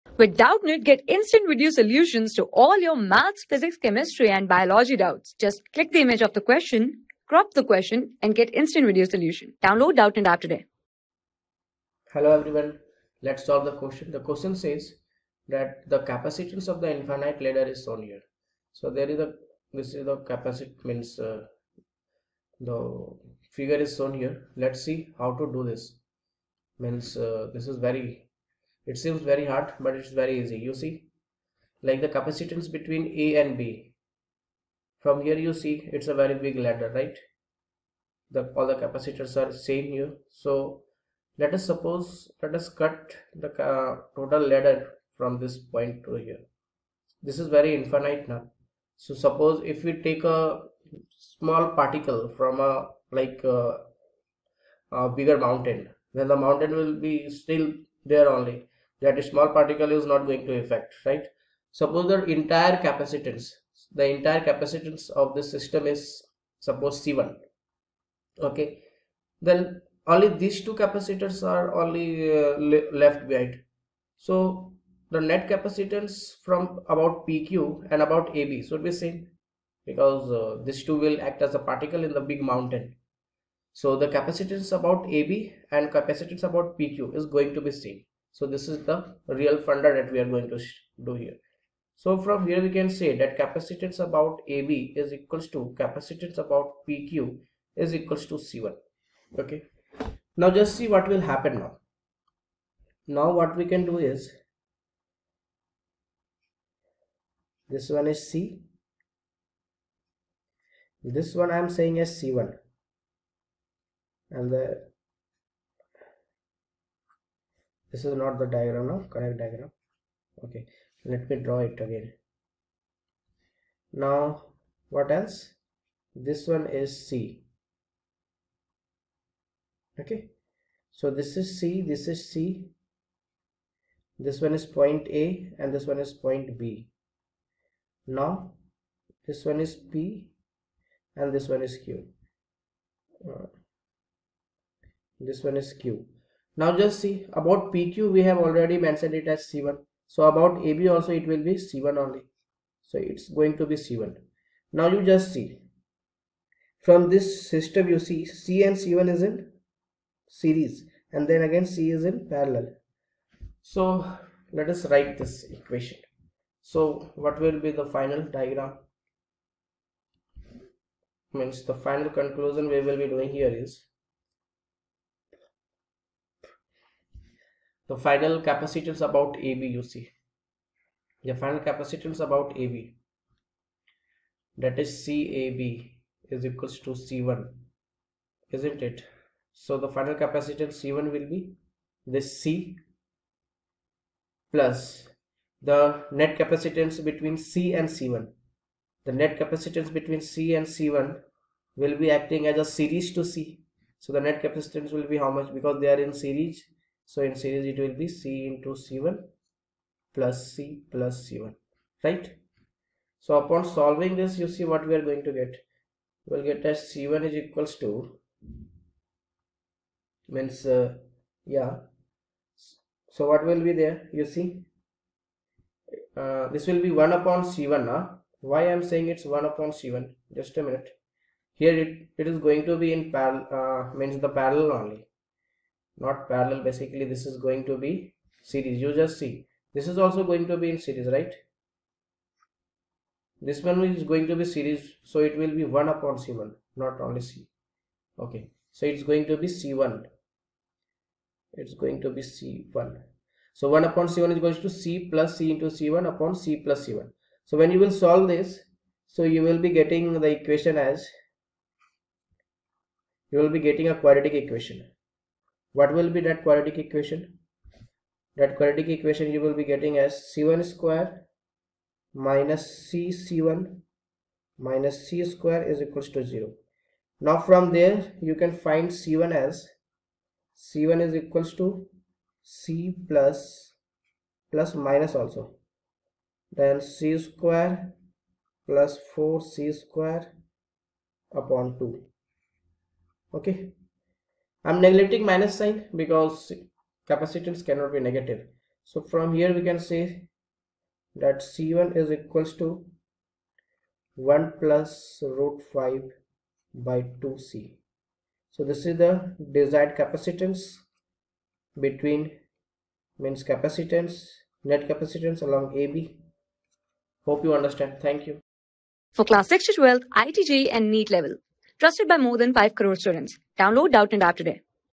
Find the capacitance ofthe infinite ladder shown in figure. | 12 | CAPACITORS | PHYSICS | HC VER...
Find the capacitance ofthe infinite ladder shown in figure.
Class: 12
Subject: PHYSICS
Chapter: CAPACITORS
Board:IIT JEE

👉 Have Any Query? Ask Us.